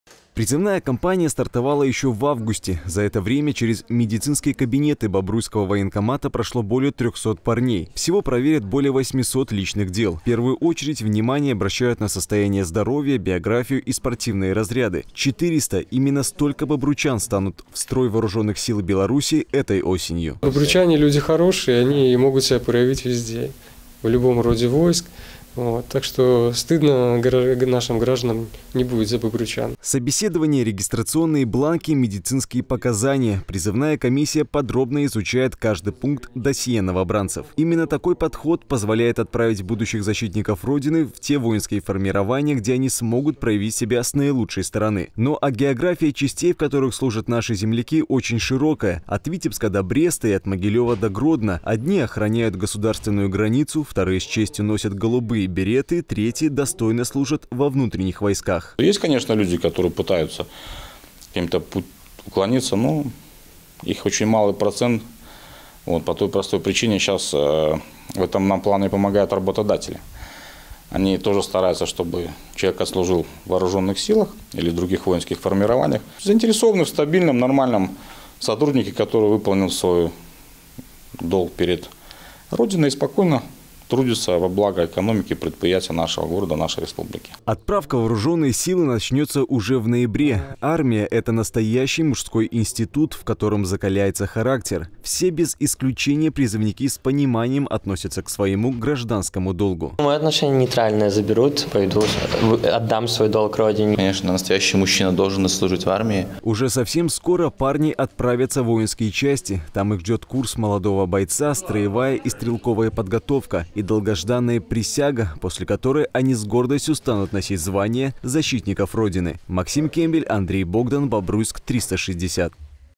Дело чести
В ноябре, защитниками Родины, станут более четырехсот бобруйчан. Наши земляки всегда претендуют на места в самых элитных войсках.